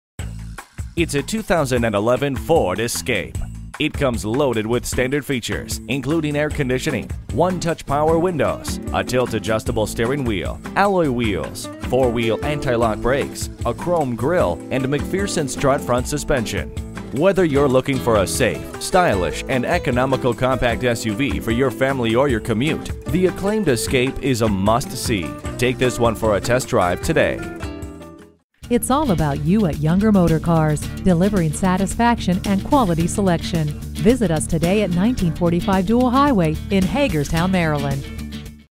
2011 Ford Escape Martinsburg-WV Hagerstown, MD #7296900 SOLD
Year: 2011
Make: Ford
Model: Escape
Trim: Limited
Engine: 6 Cyl. 3.0L
Transmission: Automatic
Color: Tuxedo Black Metallic
Mileage: 55565
Stock #: 7296900
One owner 4x4 Escape. Clean carfax and history report.

Address:
1945 Dual Highway